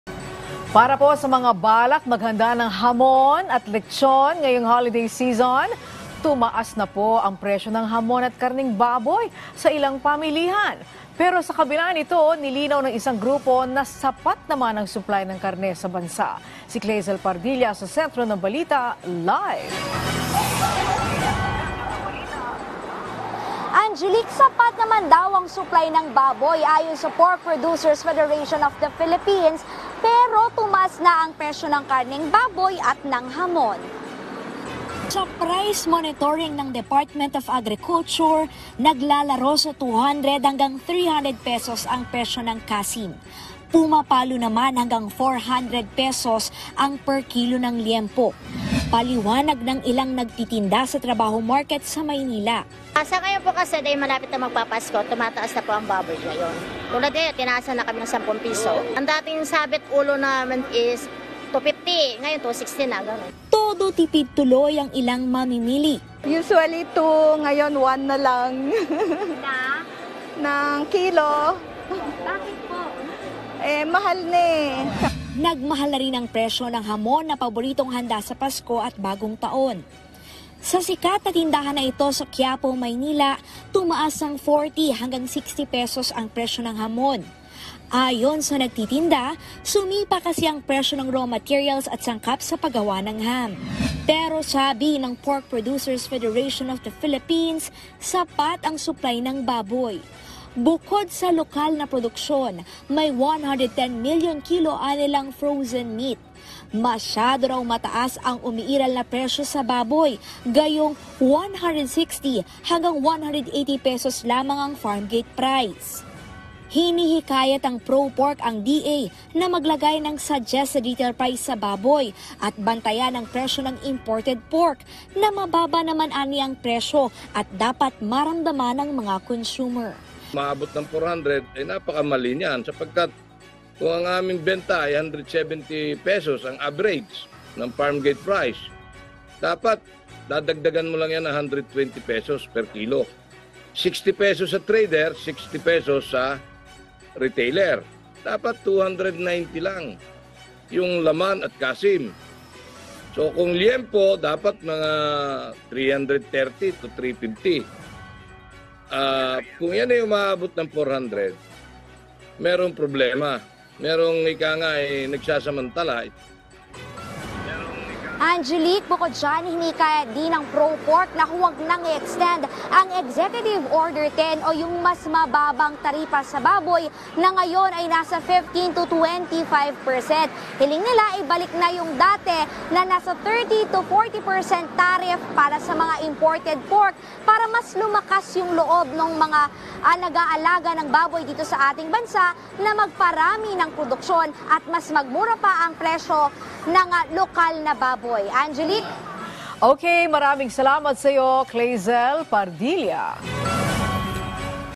Presyo ng karneng baboy at hamon, tumaas na sa kabila ng sapat na supply
1 grupo, hinikayat ang DA na magtakda ng SRP sa baboy